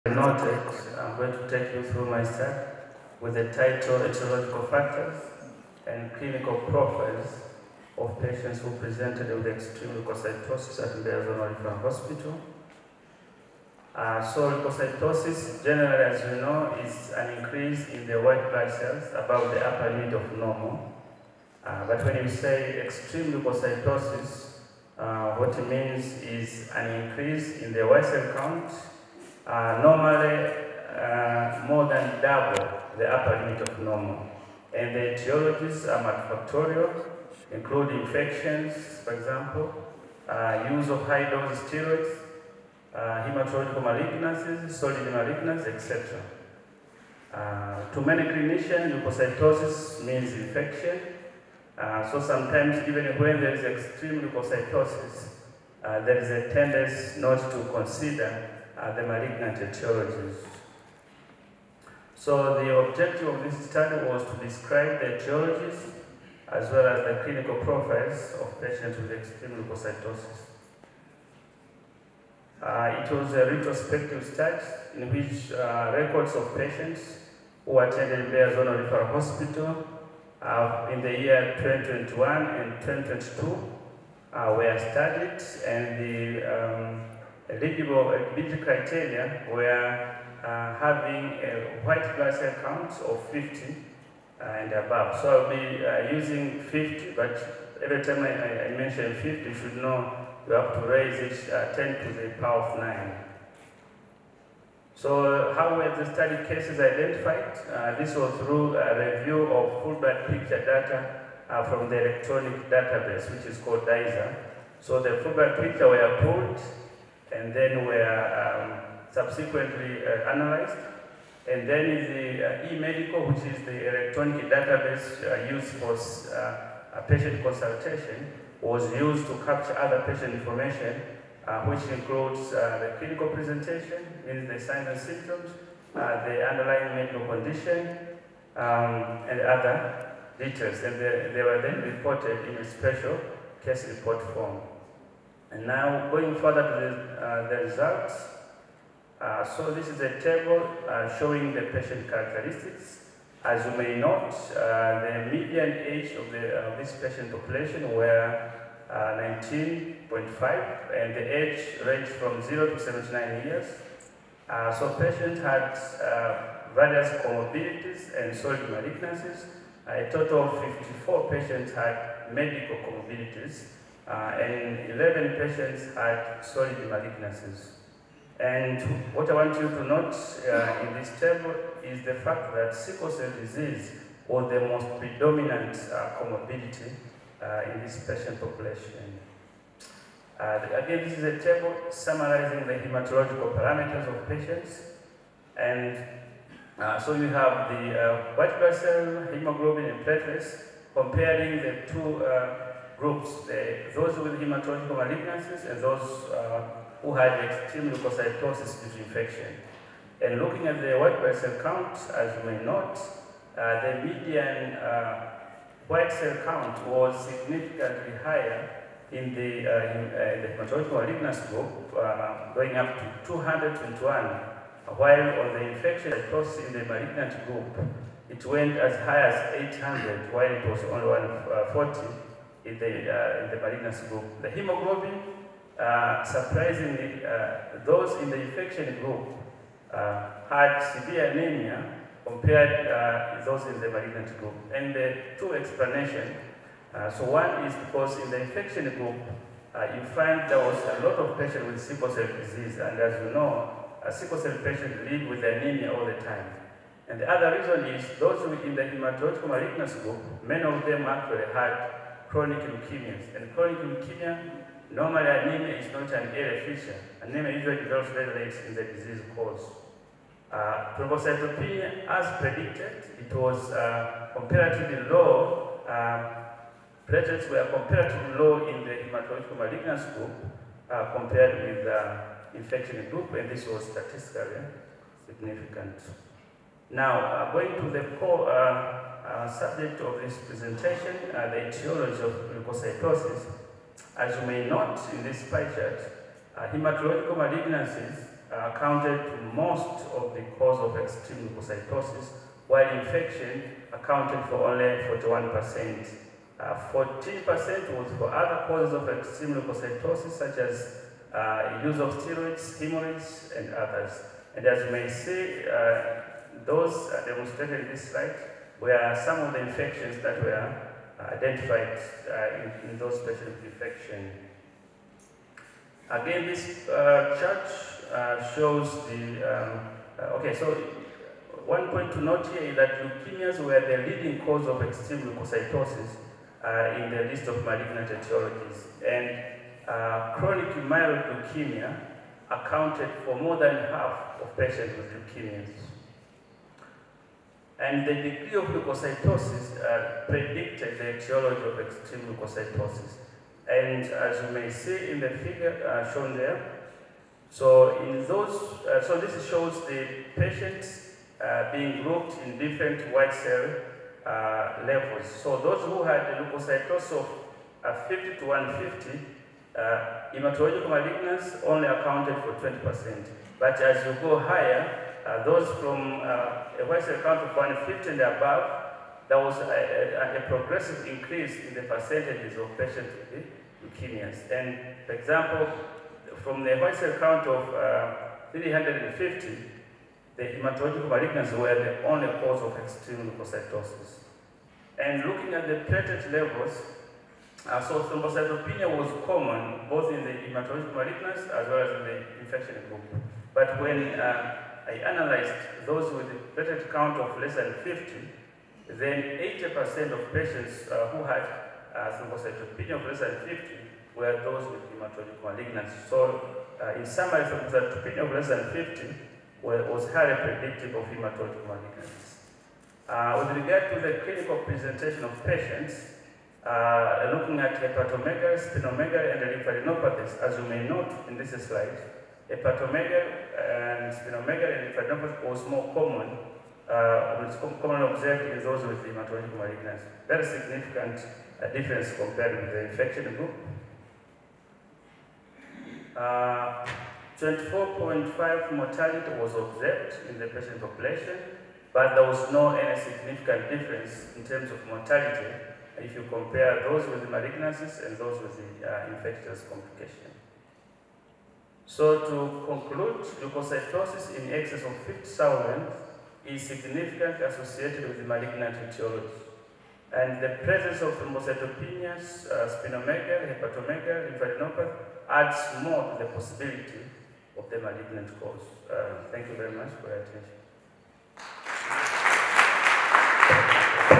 Kongamano la Kisayansi, Kitaaluma na Tafiti (Share)
Kuongezeka kwa seli nyeupe za damu mara nyingi huashiria uwepo wa maambukizi ya vijidudu vya magonjwa. Hata hivyo ongezeko hilo linapozidi mara dufu chanzo chake kinaweza kuwa ni matatizo mengine makubwa na hatari kiafya.

Dkt. Adam Kilungu ni Daktari Bingwa wa Magonjwa ya damu kutoka Hospitali ya Rufaa ya Kanda Mbeya  katika wasilisho lake ameonyesha ongezeko  la seli nyeupe za damu kuanzia 50 x 10^9/L au zadi linatokea zaidi kwa wagonjwa ambao wanakuwa na saratani.

Uwezekano wa saratani unakuwa mkubwa zaidi pale ambapo ongezeko la seli nyeupe linaambatana na kuvimba kwa ini, bandama au tezi mbali mbali. Dkt Adam amefikia hitimisho hilo kufuatia utafiti alioufanya uliohusisha wagonjwa 178 waliotibiwa Hospitali ya Rufaa ya Kanda Mbeya katika mwaka 2021 na 2022.

Kati ya wagonjwa 178, wagonjwa 84 (45%) ongezeko la seli nyeupe lilisababishwa na saratani. Saratani ya damu ndiyo ilitawala zaidi kwa kuwa na wagonjwa 89%.

Kufuatia matokeo haya Dkt Adam anashauri madaktari kuwachunguza kwa makini wagonjwa wanaogundulika kuwa na seli nyeupe zaidi ya 50 x 10^/L ili kubaini kama wana saratani au la.